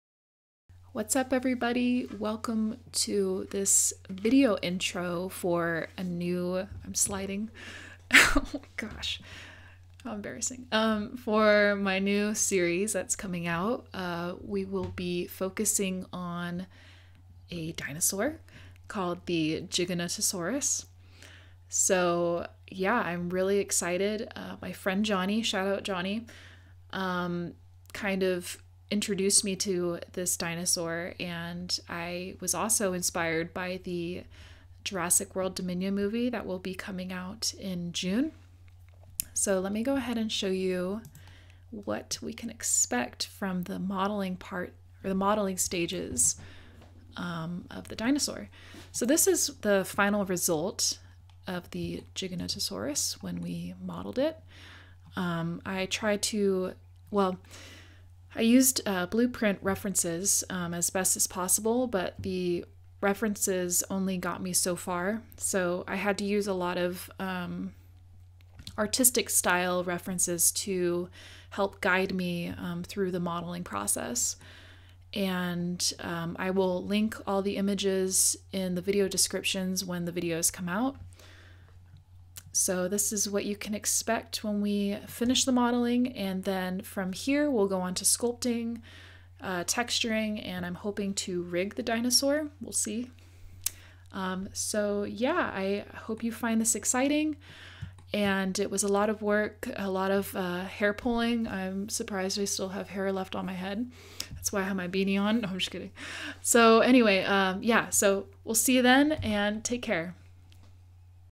NEW DINO SERIES: Modeling Reveal Introduction (Blender)
Hey everybody!

Welcome to this very awkward video intro for the new dinosaur series that will be coming out soon!

The dinosaur that we will begin focusing on is the Giganotosaurus. I hope you are able to learn something new from the videos. As stated, I will provide all the references that you will need to begin modeling your Giganotosaurus.

As always, please leave any questions you might have. I'm looking forward to seeing you in the first stage of modeling the dino and I hope you enjoy the series! :)